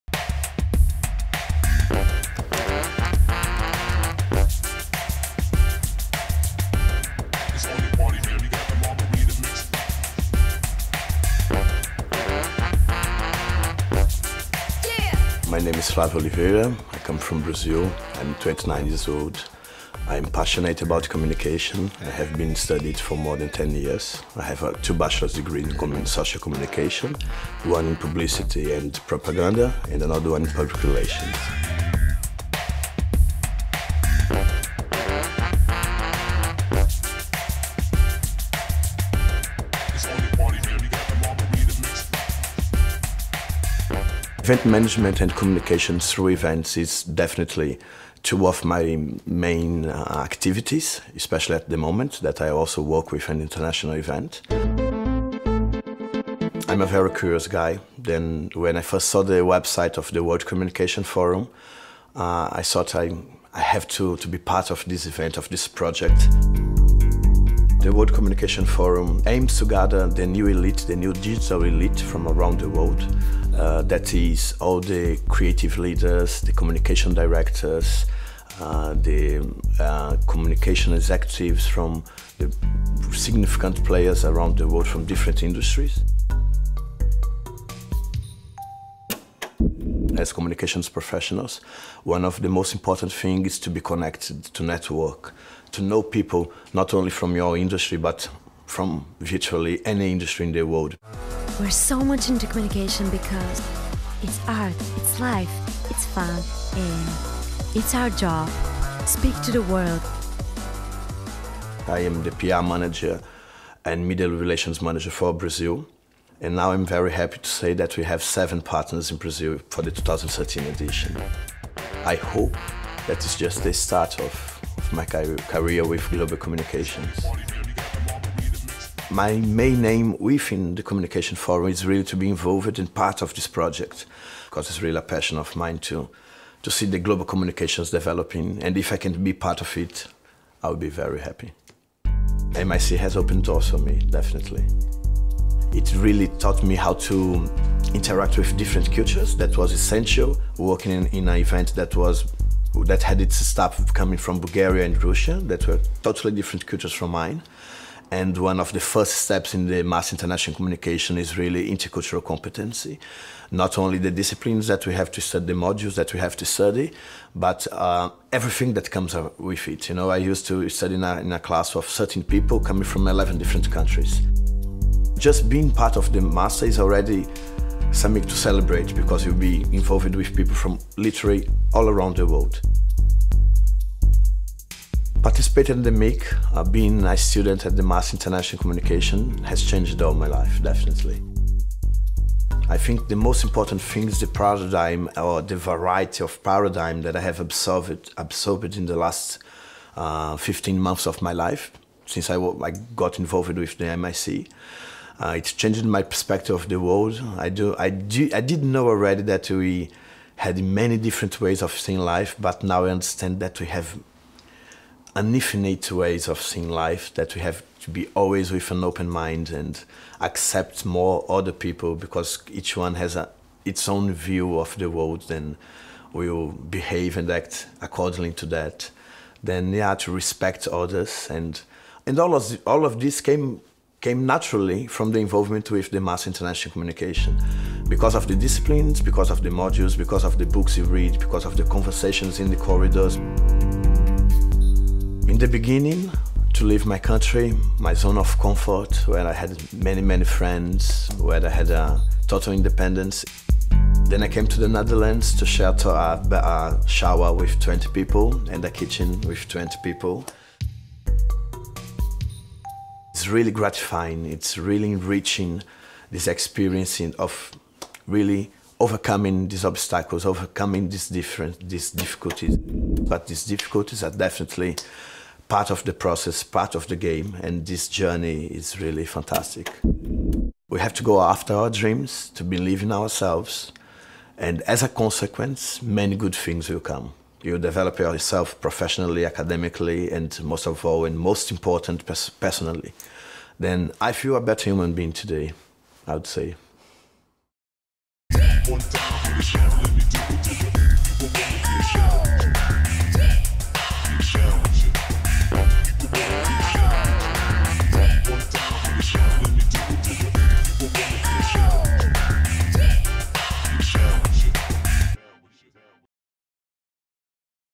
Life as a MIC Graduate - Flavio Oliveira
This is the third of a series of Master International Communication (MIC) graduate portraits: Flavio Oliveira, about working for the World Communication Forum and his time as a MIC student.

Film by Siegmund Audiovisuele Produkties.